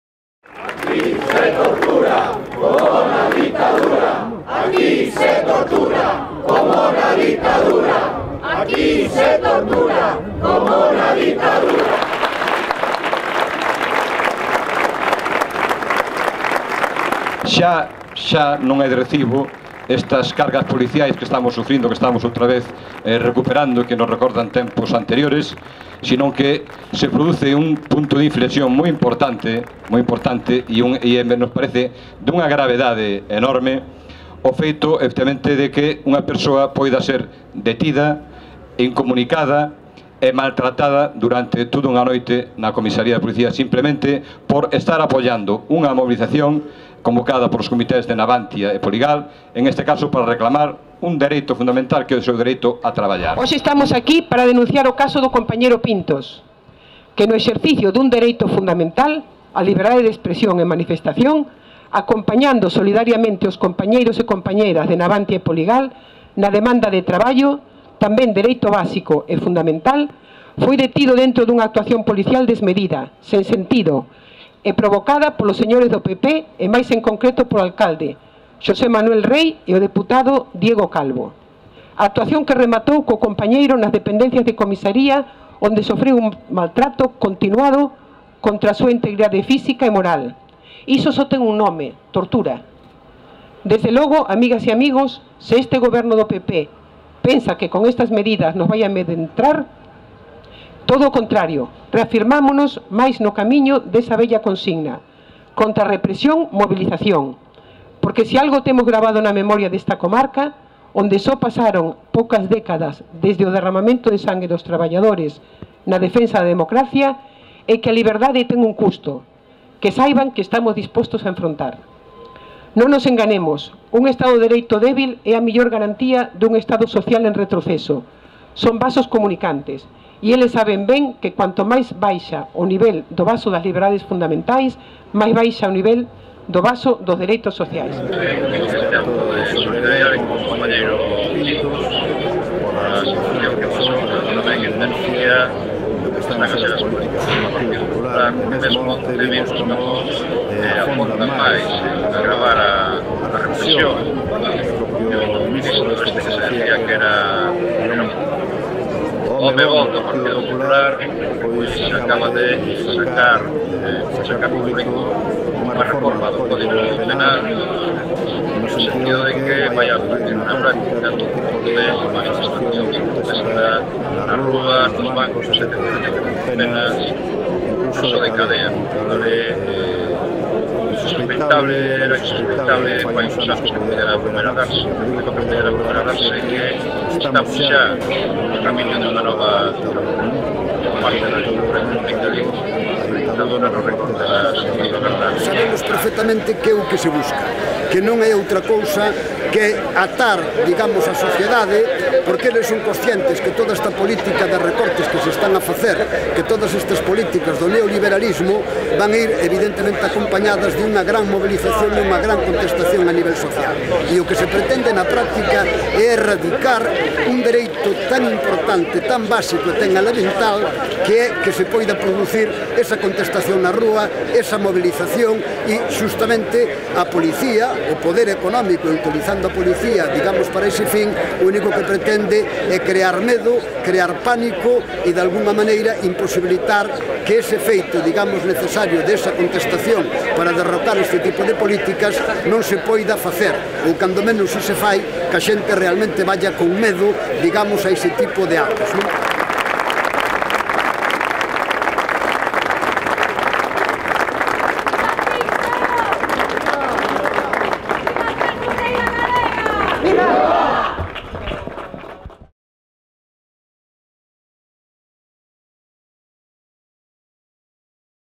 Concentración en solidariedade con Pintos, torturado pola policía en Ferrol (18/10/12)
Concentración en solidariedade co secretario comarcal da CIG en Ferrol, X.A López Pintos, torturado pola policía en Ferrol despois de ser detido durante un protesto dos traballadores do naval (18/10/12)